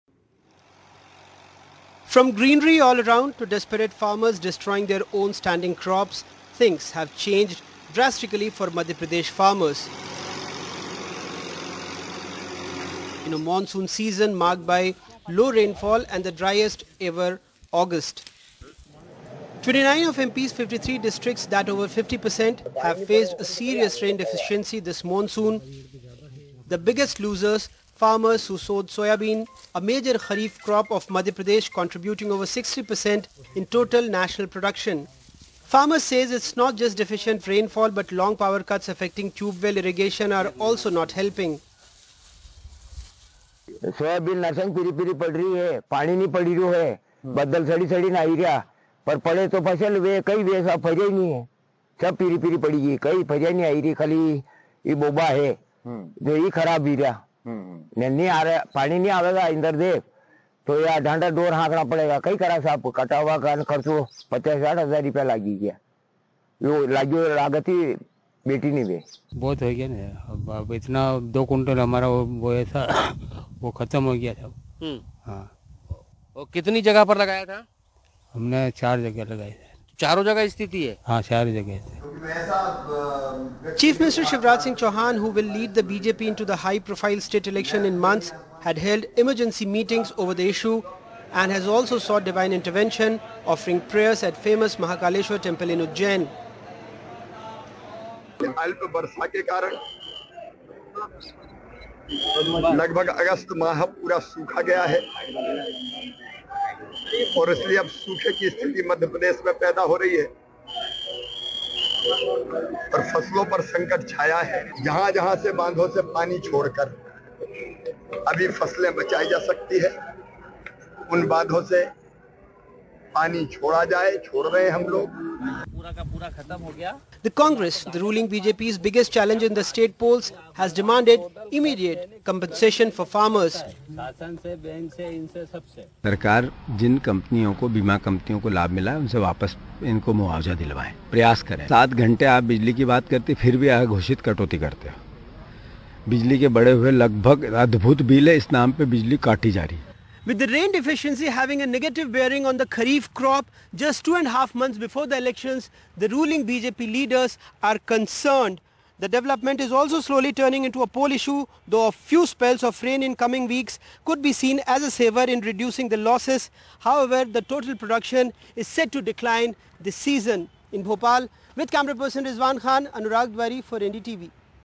Madhya Pradesh Soybean Farmers In Distress
With Deficient Rain Casting A Severely Adverse Shadow On The Kharif Crops, Particularly Soybean, This Is Slowly Turning Into A Poll Issue In The Election-bound State. According To The India Meteorological Department The Entire State As Of September 3, 2023, Has Reported 18 Percent Below Normal Rain This Monsoon And An Extremely Dry August.